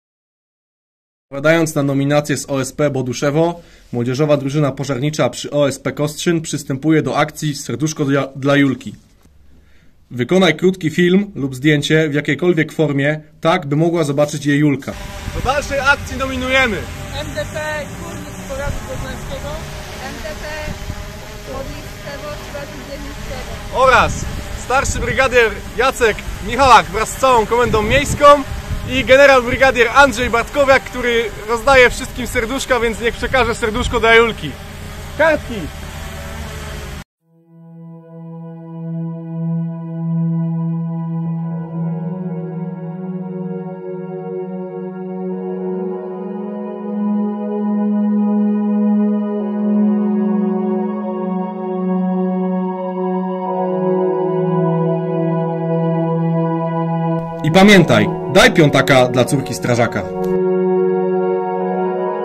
MDP Kostrzyn dla Julki
Zostaliśmy poproszeni o nagranie filmu z MDP Kostrzyn:

❤️SERDUSZKO DLA JULKI❤️

Córka strażaka z OSP Mrowino potrzebuje naszej pomocy.
"Jeden za wszystkich, wszyscy za jednego "

Do udziału w akcji nominujemy również:
MDP  Ochotnicza Straż Pożarna w Kórniku
MDP Ochotnicza Straż Pożarna Modliszewo

Oraz

St bryg. Jacek Michalak wraz z całą Komenda Miejska Państwowej Straży Pożarnej w Poznaniu
Gen. bryg. Andrzej Bartkowiak- który na Twitterze zawsze rozdaje serduszko - prosimy by przekazał również serduszko dla małej Juleczki.

Wykonaj zdjęcie lub krótki film zawierający SERDUSZKO (w jakiejkolwiek formie), tak aby mogła je zobaczyć Julka.

"Daj piątaka dla córki strażaka" - wpłać wielokrotność 5zl za każdego członka MDP na konto zbiórki